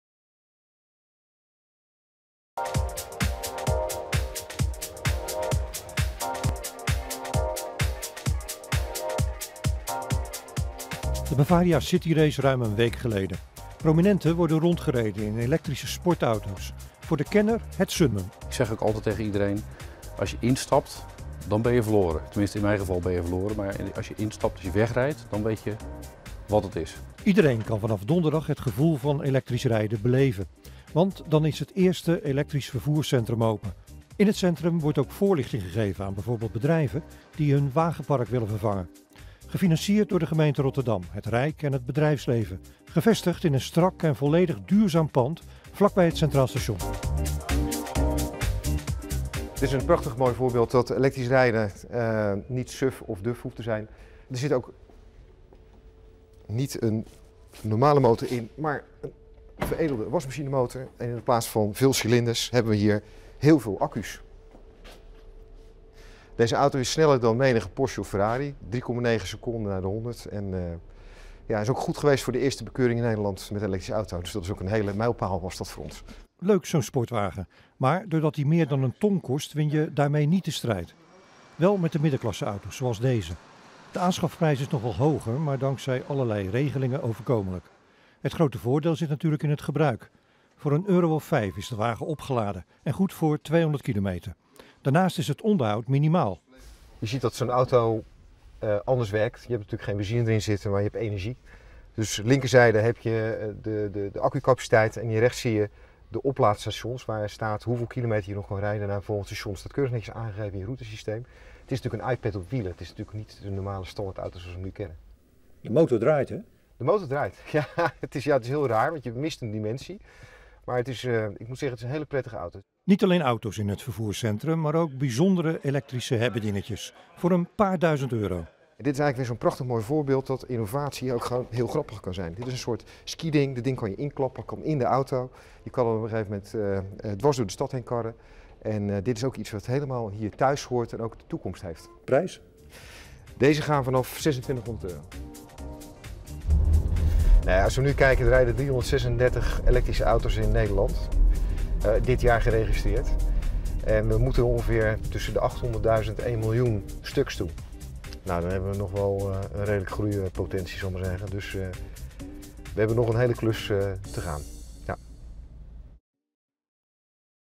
Minister opent het Elektrische Vervoercentrum
Minister Schultz van Haegen van Milieu opent morgen in Rotterdam het Elektrische Vervoercentrum. Bijzonder, want het is de eerste ter wereld! Het centrum moet burgers en bedrijven overtuigen van nut, noodzaak en gemak van electrische vervoersmiddelen.